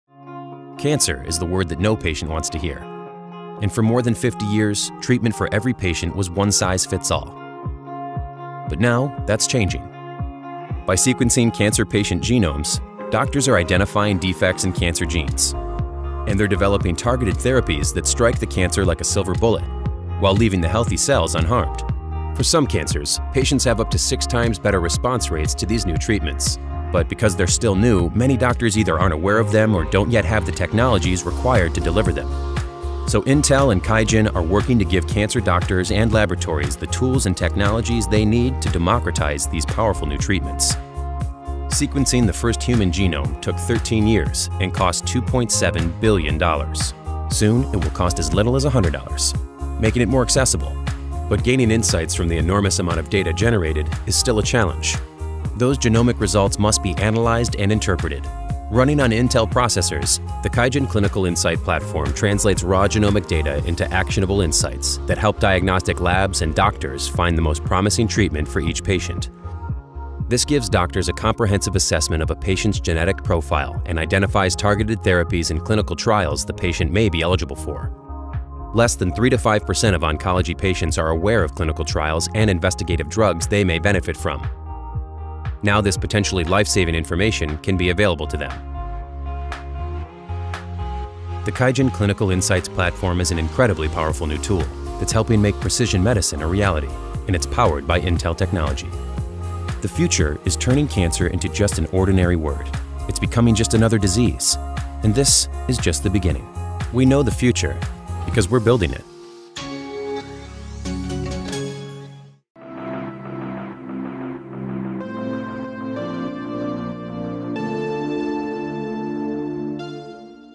Data and DNA Fight Disease | Intel Business
The healthcare solutions of the future use an individual’s genes and high-powered computing to produce personalized treatments for each patient, identify genetic defects causing different cancers, and develop targeted therapies that kill harmful cells while leaving healthy ones intact.